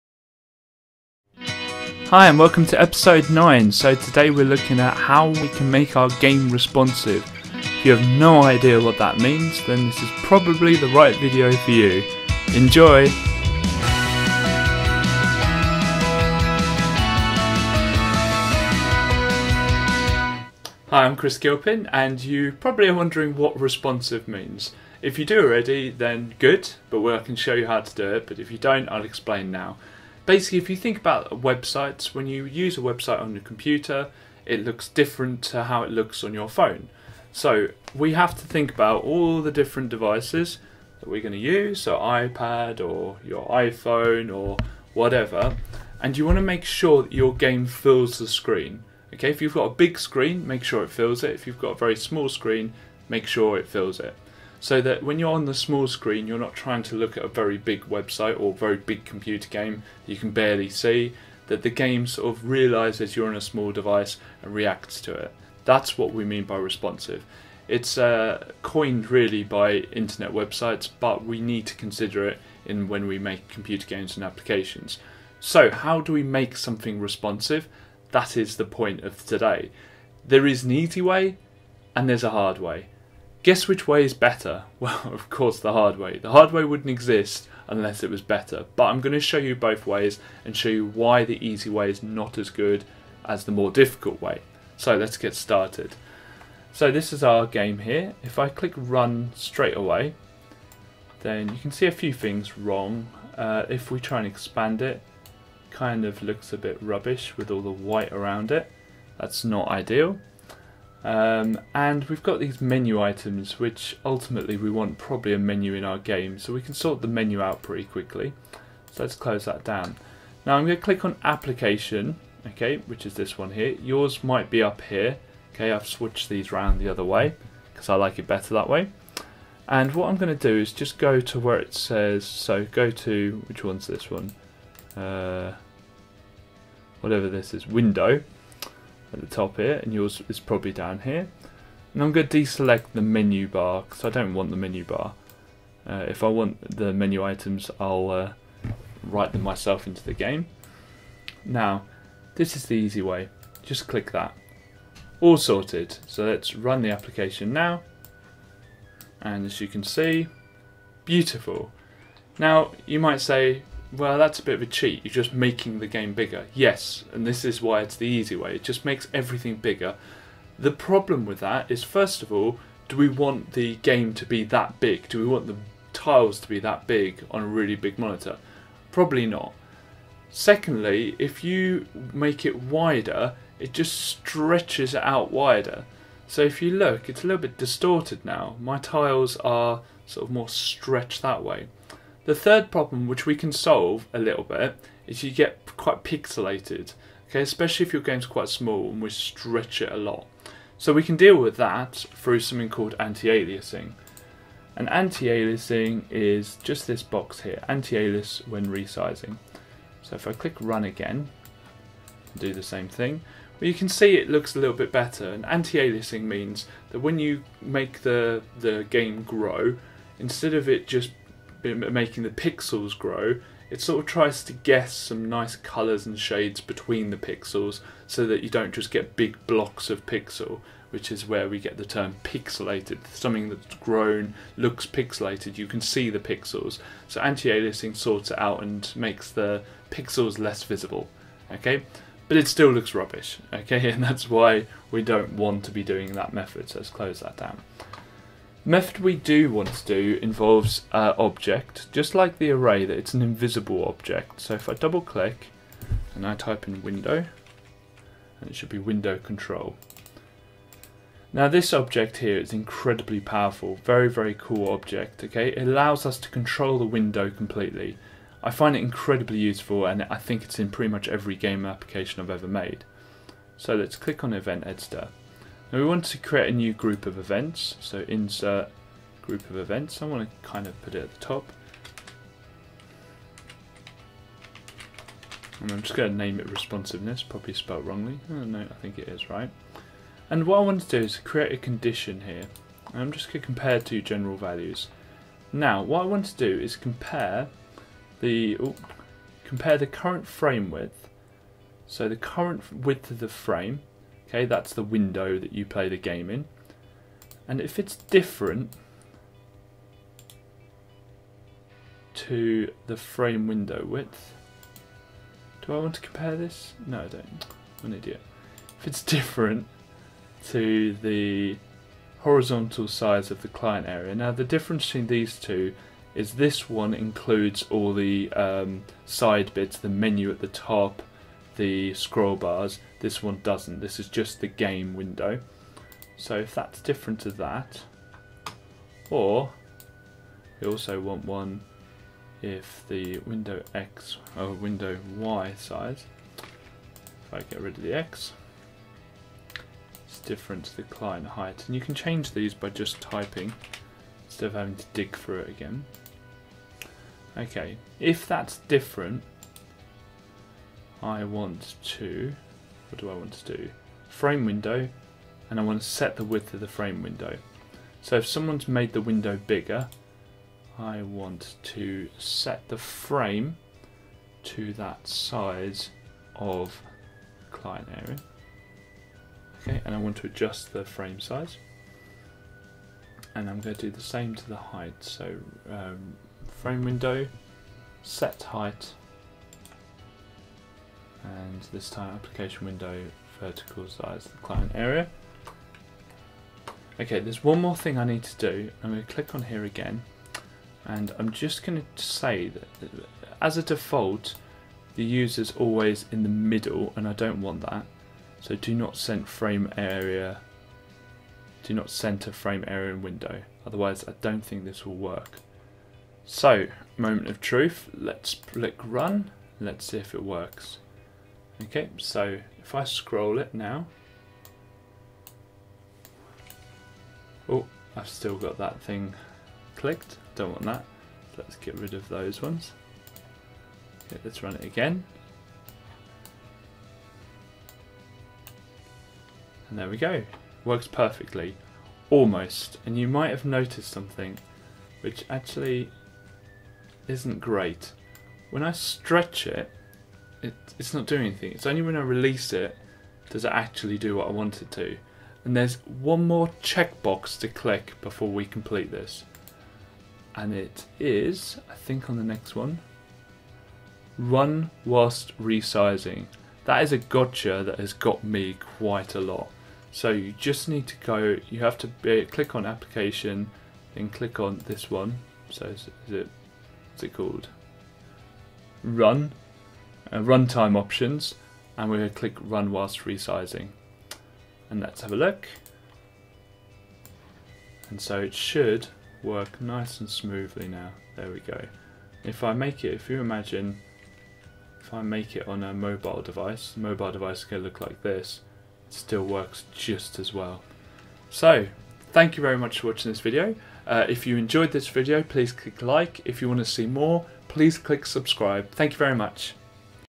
Create an RPG - Clickteam Fusion 2.5 - Pt 9 (Clickteam Tutorial)
Create an RPG game with Clickteam Fusion 2.5 Part 9.

We look at making our game responsive so that it fills the whole window, on any device. We also get rid of the menu at the top.